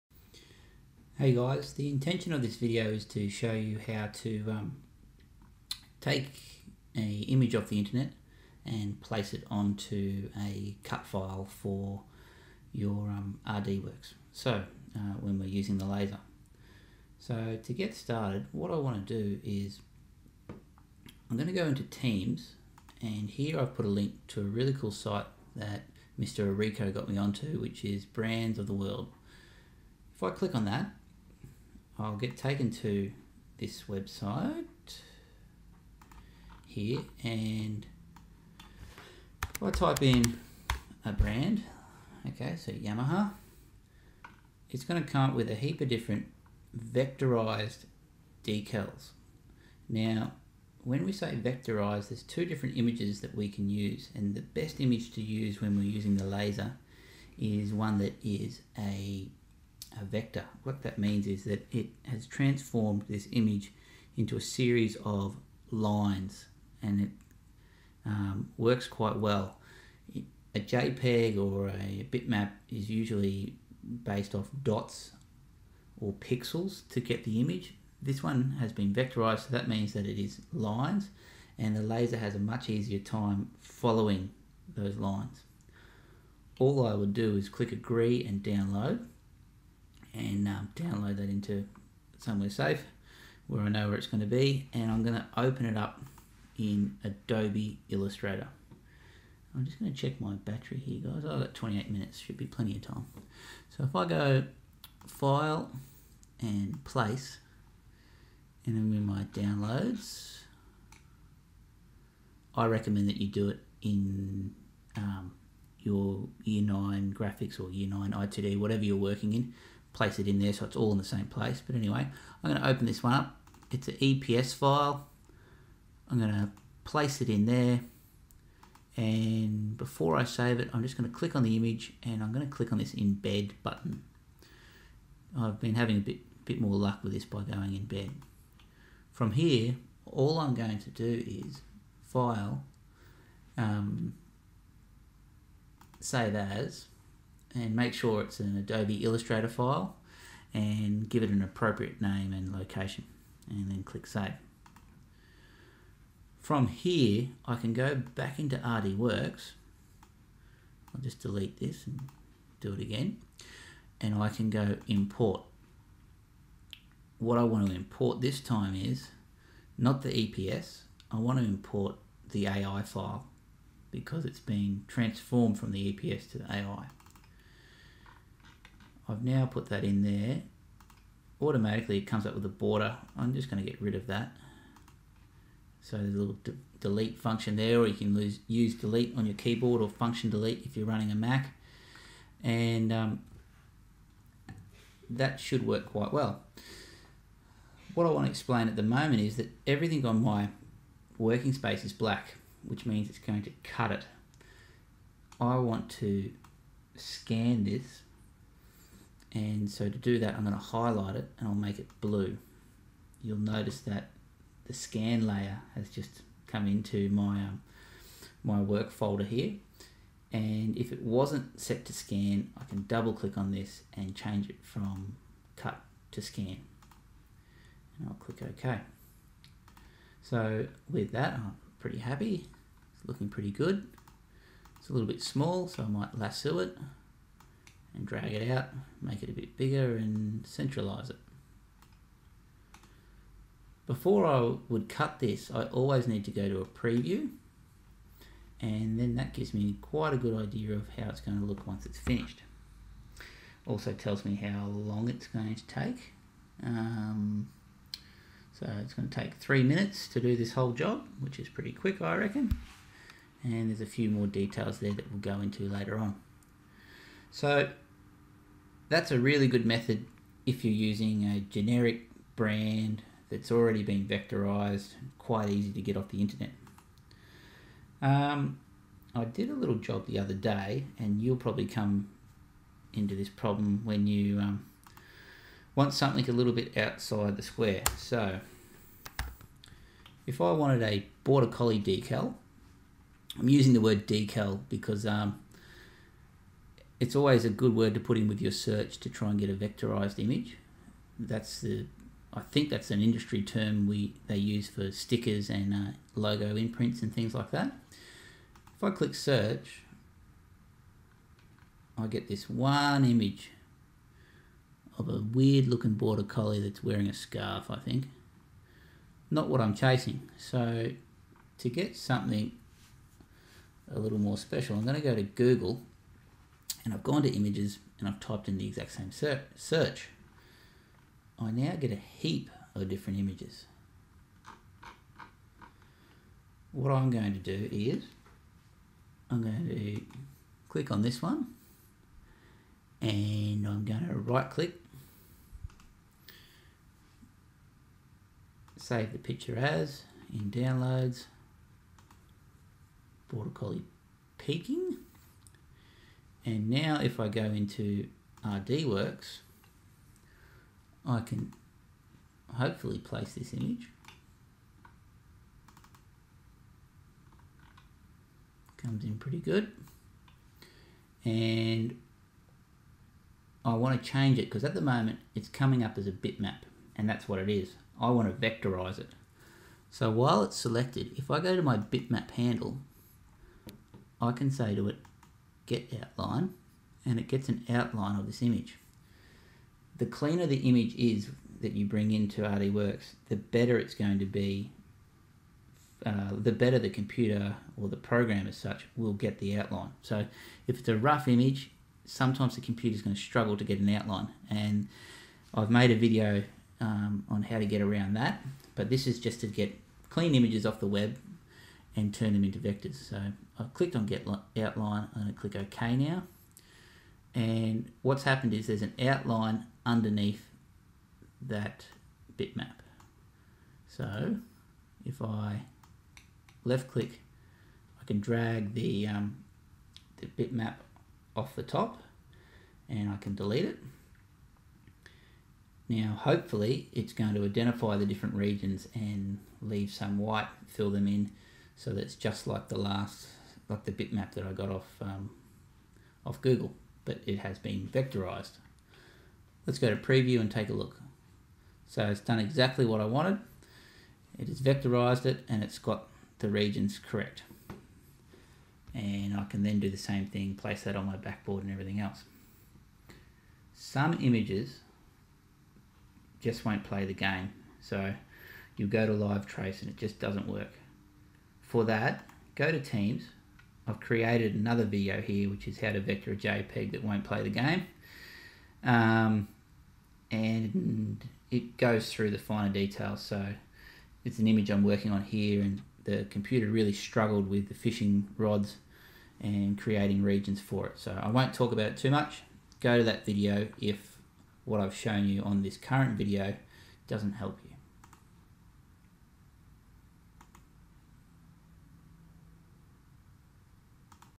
Placing Vectors into RD works & make a Vector
How to take a vector from the net and place it into RD works, and also how to trun a jpeg into a vector using "get outline"